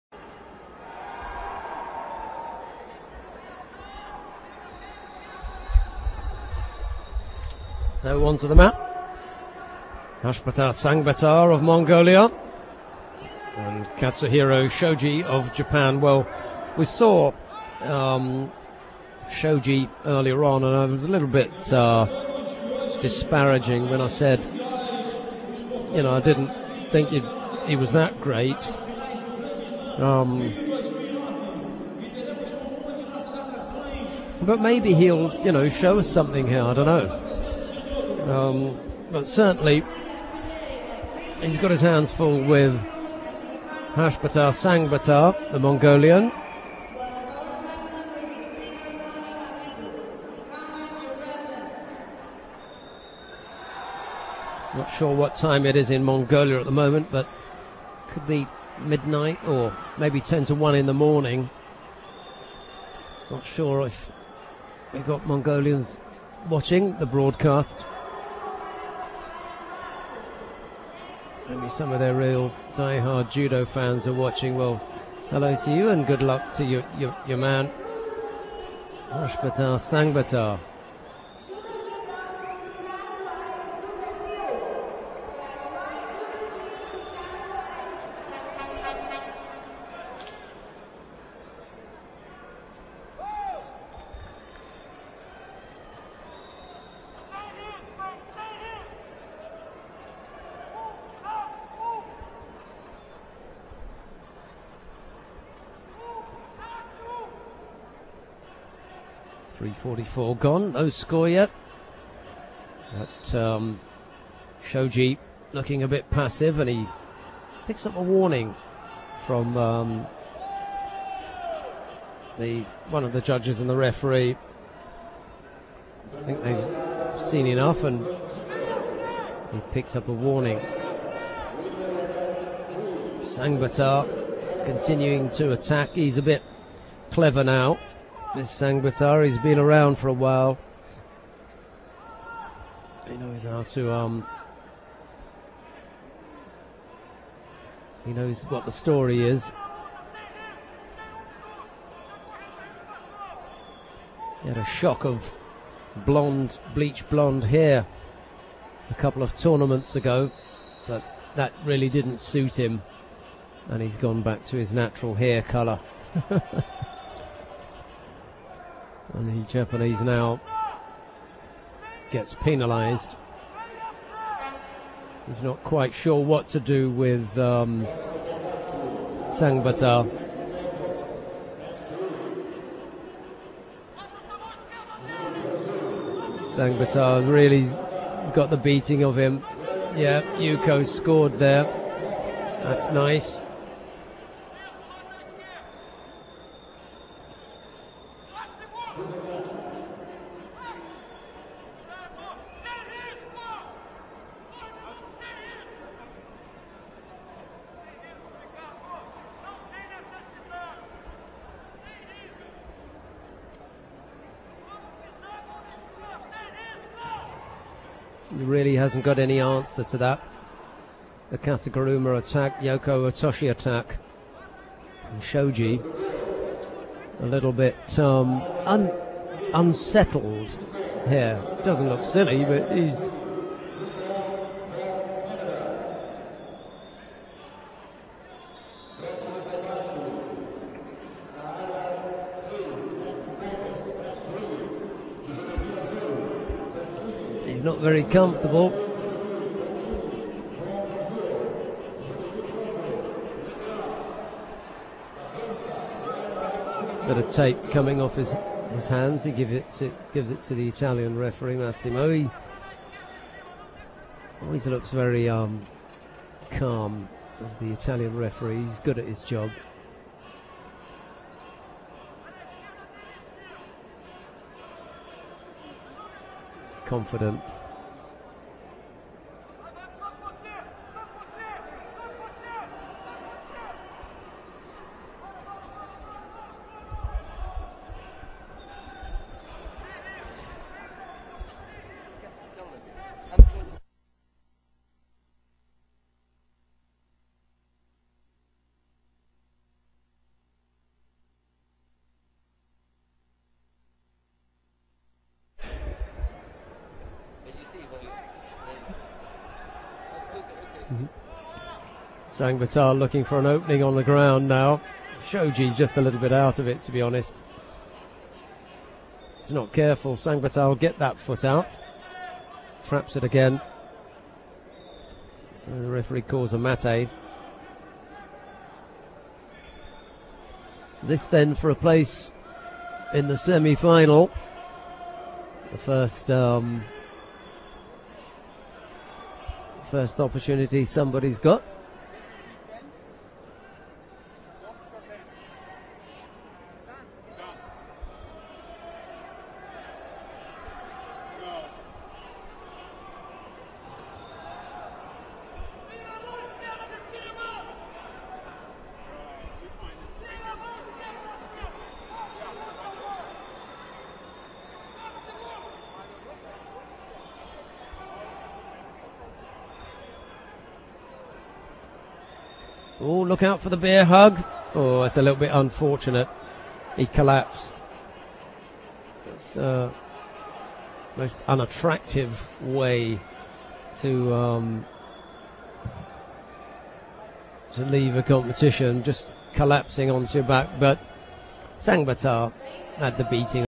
Judo 2009 Rio de Janeiro: Tsagaanbaatar (MGL) - Shoji (JPN) [-66kg].
Judo 2009 Grand Slam Rio de Janeiro
Khasbaatar Tsagaanbaatar (MGL) - Kazuhiro Shoji (JPN) [-66kg].